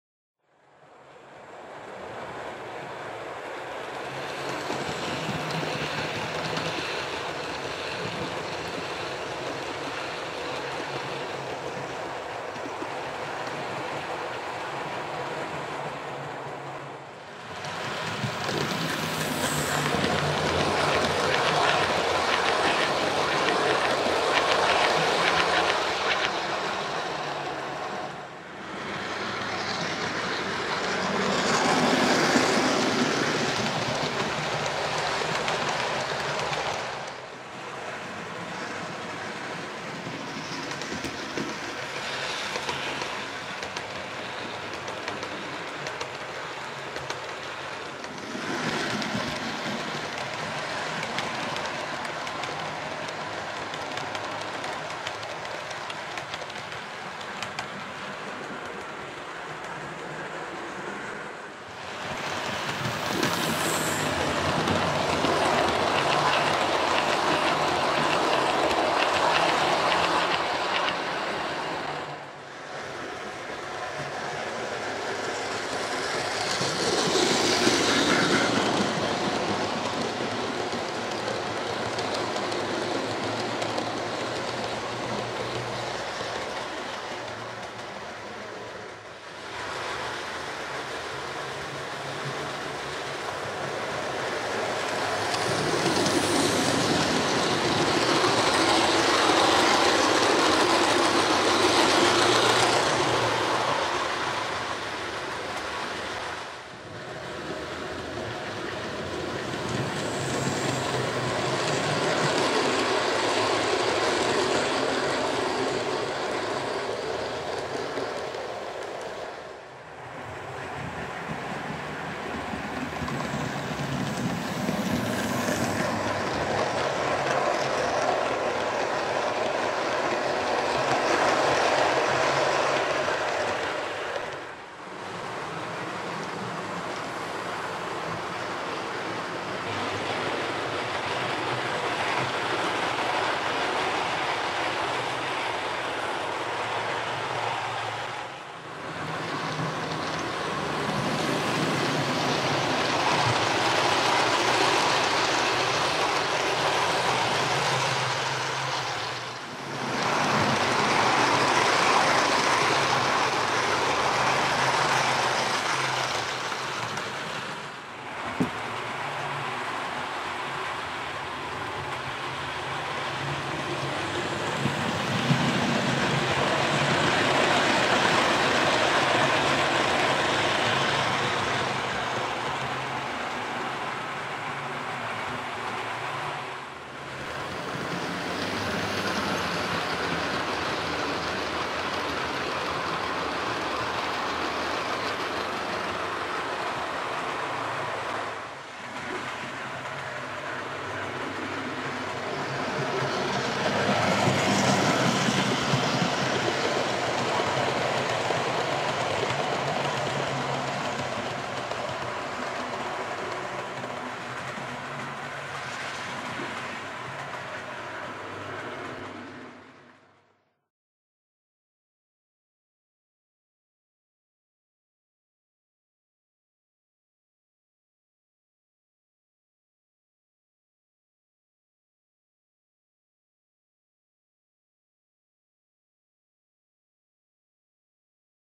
Midland Express.
LMR passenger action on Sumware featuring a recent addition to my loco stable, a Bachmann Stanier Jubilee class "Hong Kong"